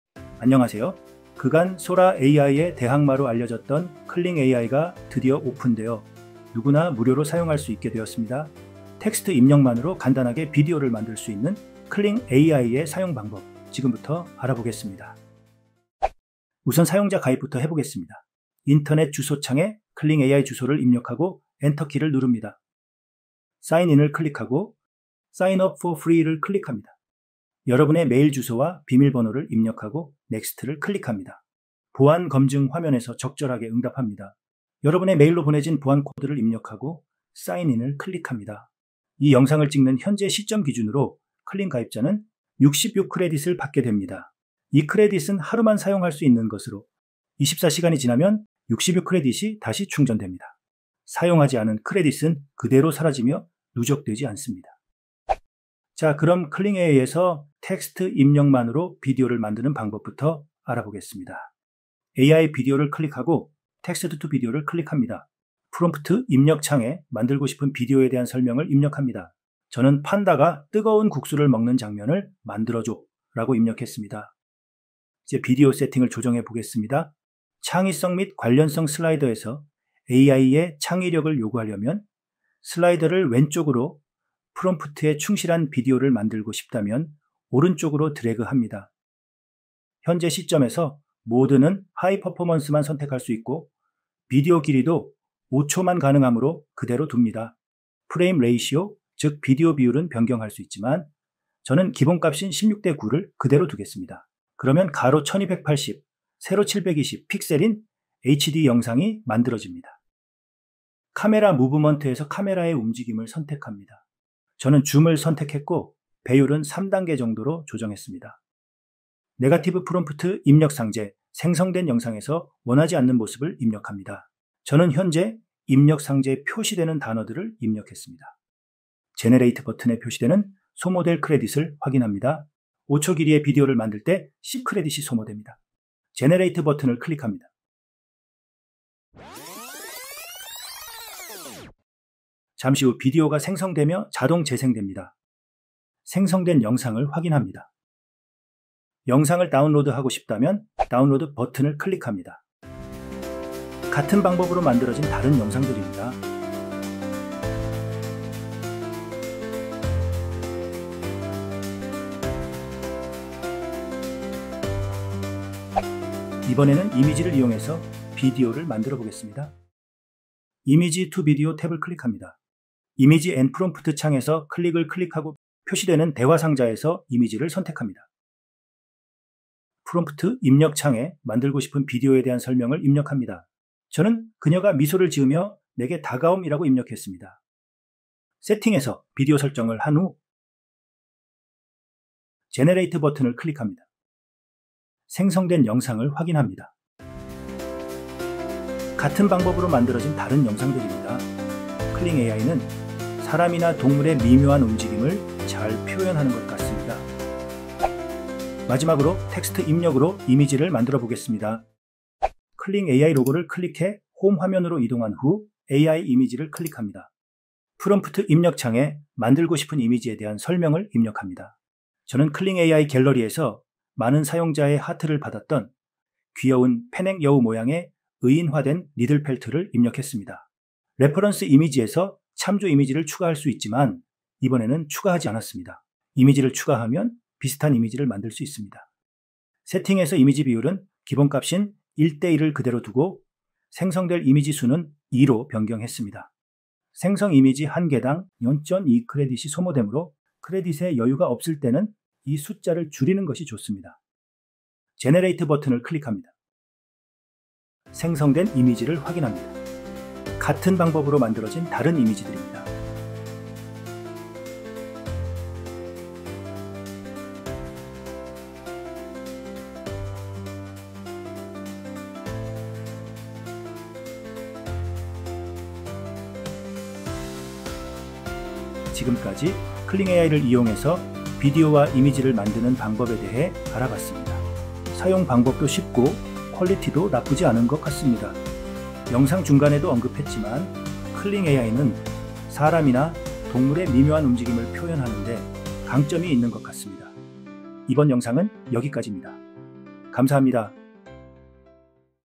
Kling AI 완벽 가이드
Sora AI 대항마로 알려진 Kling AI가 무료로 공개되어, 텍스트 입력만으로 비디오를 만들 수 있게 되었습니다. Kling AI의 사용방법을 소개합니다.

[목차]
00:00 오프닝
00:16 사용자 가입하기
00:55 Text to Video(텍스트 입력으로 비디오 만들기)
02:46 Image to Video(이미지 업로드로 비디오 생성하기)
03:28 Text to Image(텍스트 입력으로 이미지 만들기)
04:38 클로징